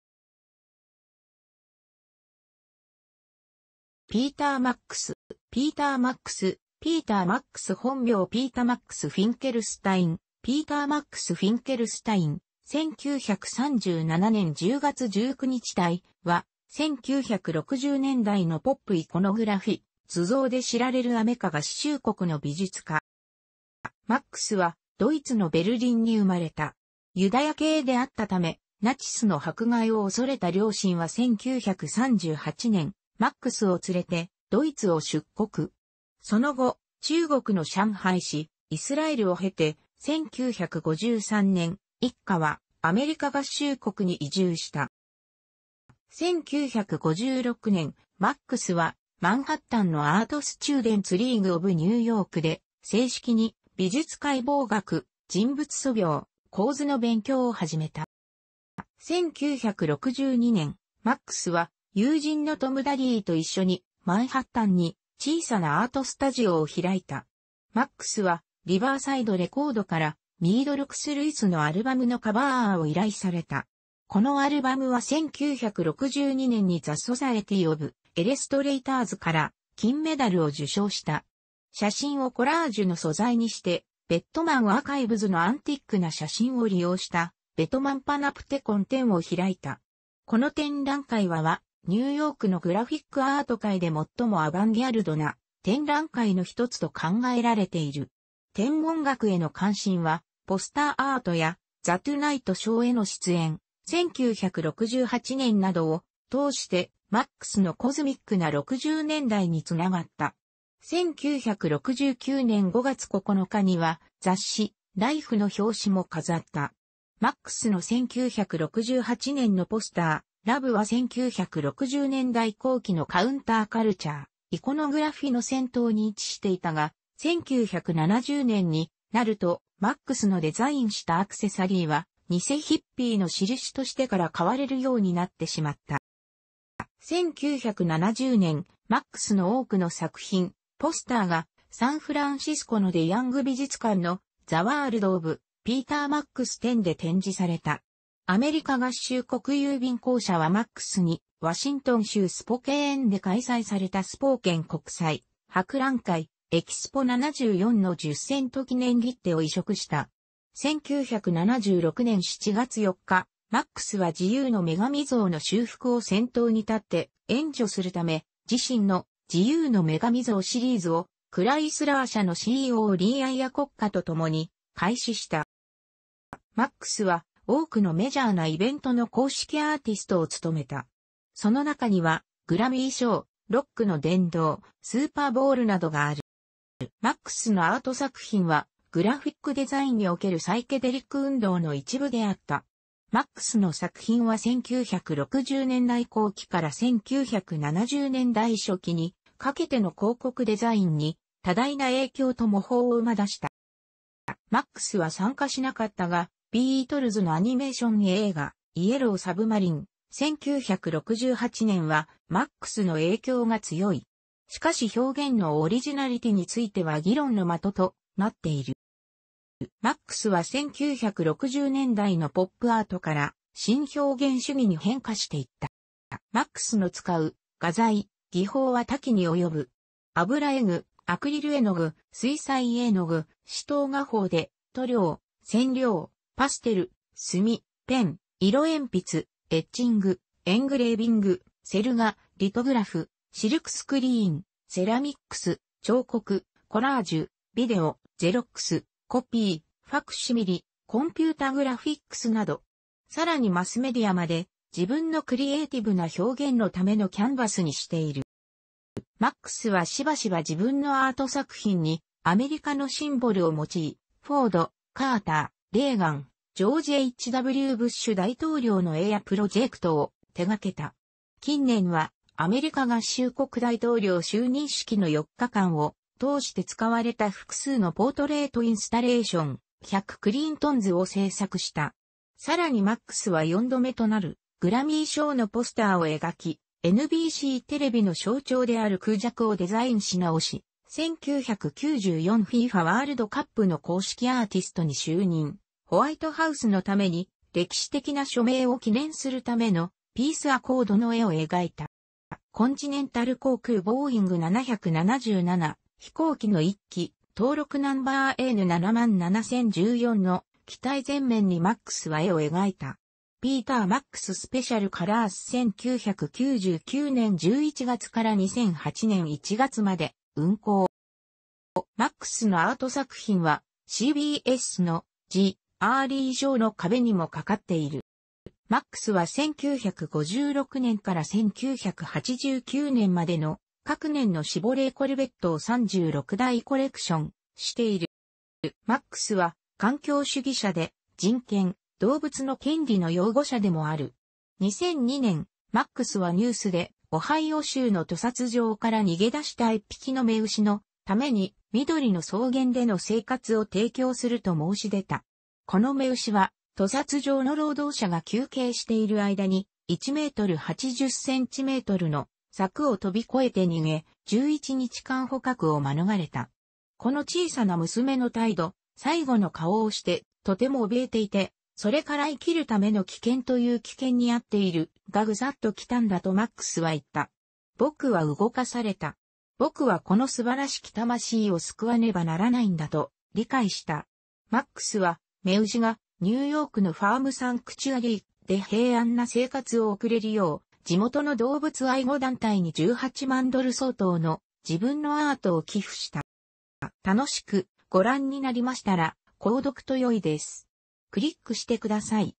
ピーター・マックス
ポップアート・アーティスト
サイケデリック・アーティスト
ナチス・ドイツから逃れたユダヤ人移民

ピーター・マックス（Peter Max）本名ピータ・マックス・フィンケルスタイン（Peter Max Finkelstein, 1937年10月19日 - ）は、1960年代のポップ・イコノグラフィ（図像）で知られるアメリカ合衆国の美術家。

マックスはドイツのベルリンに生まれた。ユダヤ系であったためナチスの迫害を恐れた両親は1938年、マックスを連れてドイツを出国。その後、中国の上海市、イスラエルを経て、1953年、一家はアメリカ合衆国に移住した。

1956年、マックスはマンハッタンのアート・スチューデンツ・リーグ・オブ・ニューヨークで正式に美術解剖学、人物素描、構図の勉強を始めた。

1962年、マックスは友人のトム・ダリーと一緒に、マンハッタンに小さなアート・スタジオを開いた。マックスはリバーサイド・レコードからミード・ルクス・ルイスのアルバムのカバー・アートを依頼された。このアルバムは1962年にthe Society of Illustratorsから金メダルを受賞した。写真をコラージュの素材にして、Bettman archivesのアンティックな写真を利用した「Bettman Panopticon」展を開いた。この展覧会ははニューヨークのグラフィック・アート界で最もアバンギャルドな展覧会の1つと考えられている。天文学への関心は、ポスター・アートや『ザ・トゥナイト・ショー』への出演（1968年）などを通してマックスのコズミックな60年代につながった。1969年5月9日には雑誌『ライフ』の表紙も飾った。マックスの1968年のポスター『Love』は1960年代後期のカウンターカルチャー・イコノグラフィの先頭に位置していたが、1970年になると、マックスのデザインしたアクセサリーは偽ヒッピーの印としてからかわれるようになってしまった。

1970年、マックスの多くの作品、ポスターがサンフランシスコのデ・ヤング美術館の「The World of Peter Max」展で展示された。アメリカ合衆国郵便公社はマックスにワシントン州スポケーンで開催されたスポーケン国際博覧会（Expo 74）の10セント記念切手を委嘱した。1976年7月4日、マックスは自由の女神像の修復を先頭に立って援助するため、自身の『自由の女神像』シリーズを、クライスラー社のCEOリー・アイアコッカとともに開始した。

マックスは多くのメジャーなイベントの公式アーティストを務めた。その中には、グラミー賞、ロックの殿堂、スーパーボウルなどがある。

マックスのアート作品はグラフィック・デザインにおけるサイケデリック運動の一部であった。マックスの作品は1960年代後期から1970年代初期にかけての広告デザインに多大な影響と模倣を生み出した。

マックスは参加しなかったが、ビートルズのアニメーション映画『イエロー・サブマリン』（1968年）はマックスの影響が強い。しかし表現のオリジナリティについては議論の的となっている。

マックスは1960年代のポップアートから新表現主義に変化していった。

マックスの使う画材・技法は多岐に及ぶ。

マックスはしばしば自分のアート作品にアメリカのシンボルを用い、フォード、カーター、レーガン、ジョージ・H・W・ブッシュ大統領の絵やプロジェクトを手掛けた。近年は、アメリカ合衆国大統領就任式の4日間を通して使われた複数のポートレイト・インスタレーション『100 Clintons』を制作した。さらにマックスは4度目となるグラミー賞のポスターを描き、NBCテレビの象徴であるクジャクをデザインし直し、1994 FIFAワールドカップの公式アーティストに就任、ホワイトハウスのために歴史的な署名を記念するための『Peace Ac...